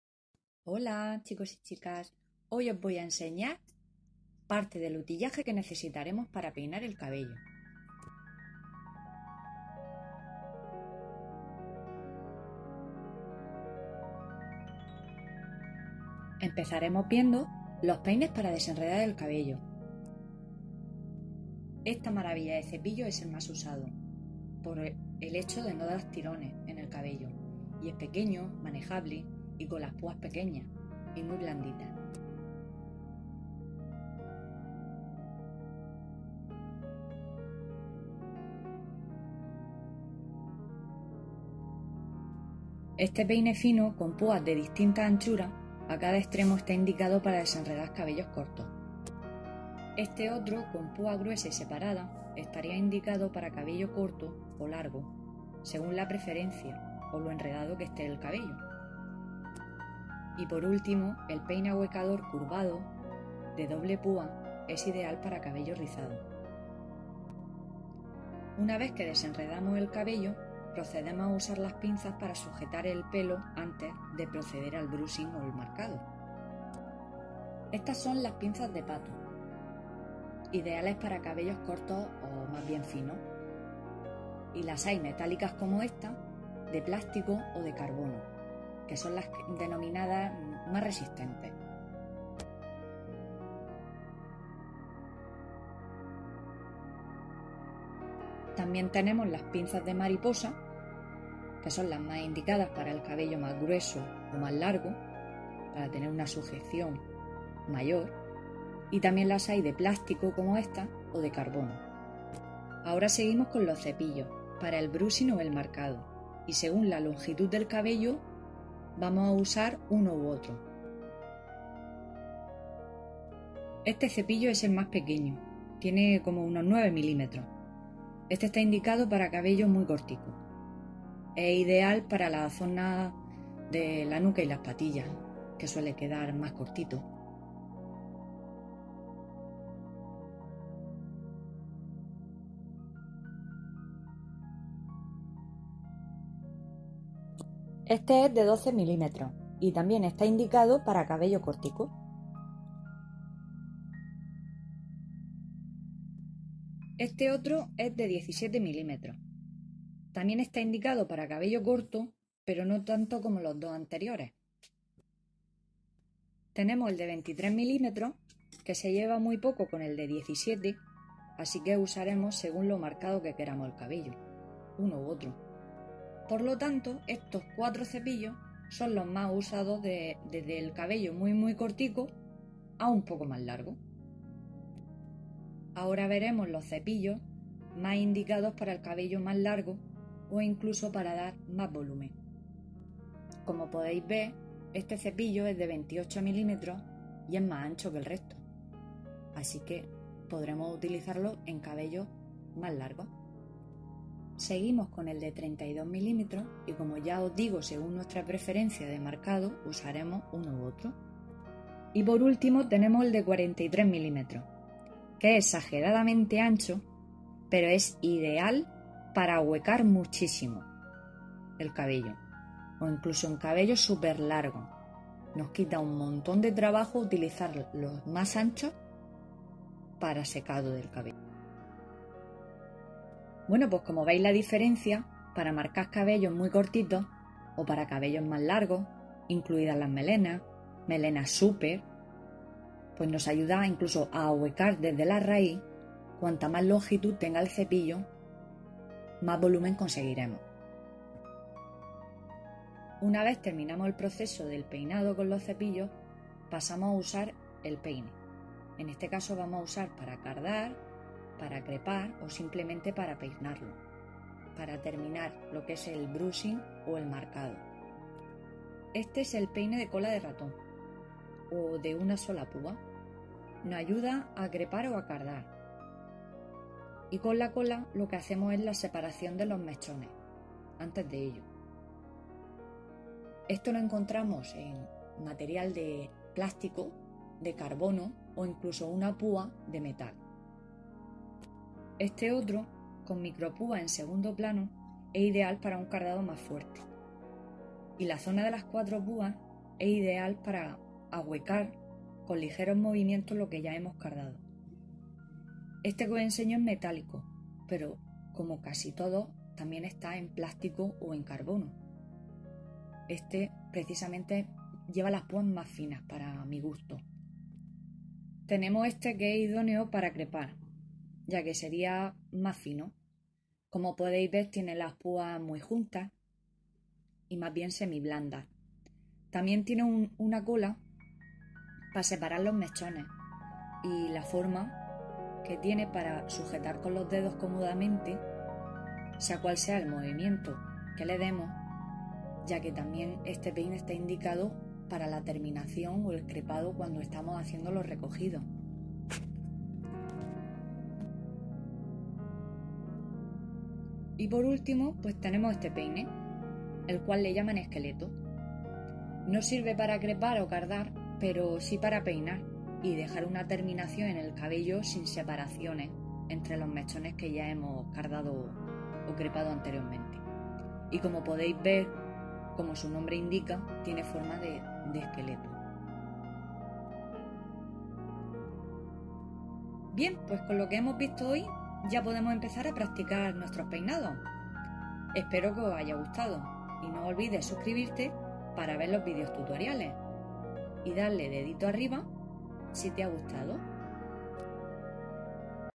Tipos de cepillos para el cabello
En este vídeo mostramos los diferentes tipos de cepillos para el cabello, el cual está relacionado con nuestro curso de peluquería gratis. @lapeluencasacursosymuchomas

Transcripción: Momentos clave:

00:35 Peines para desenredar el pelo.
02:12 Medidas de los cepillos redondos para marcar o alisar el cabello.
04:25 Tipos de peines para crepar, hacer recogidos y hacer secciones de pelo.